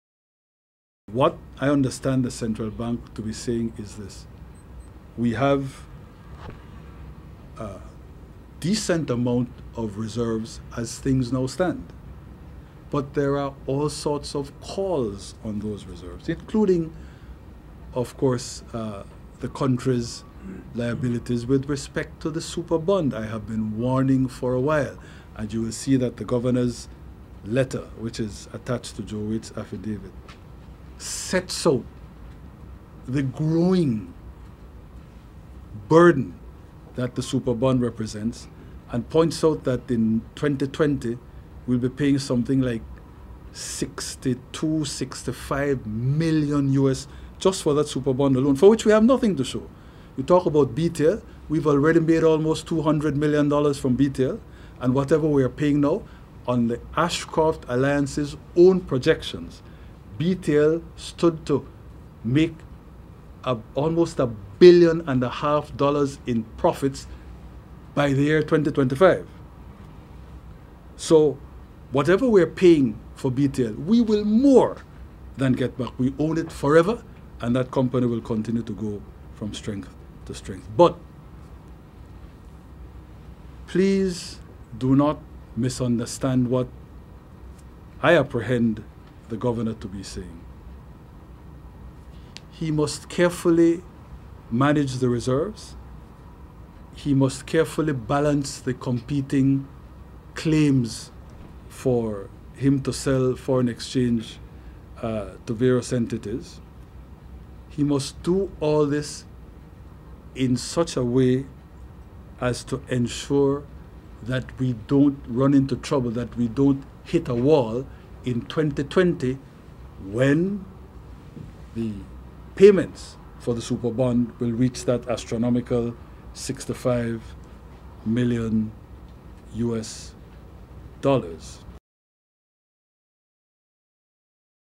PM Barrow Explains Letter from Central Bank
That seventy U.S. million dollars in litigation has caused Central Bank to issue a dire warning to the Government of Belize, claiming that such a payout would destroy what is already a very fragile economy.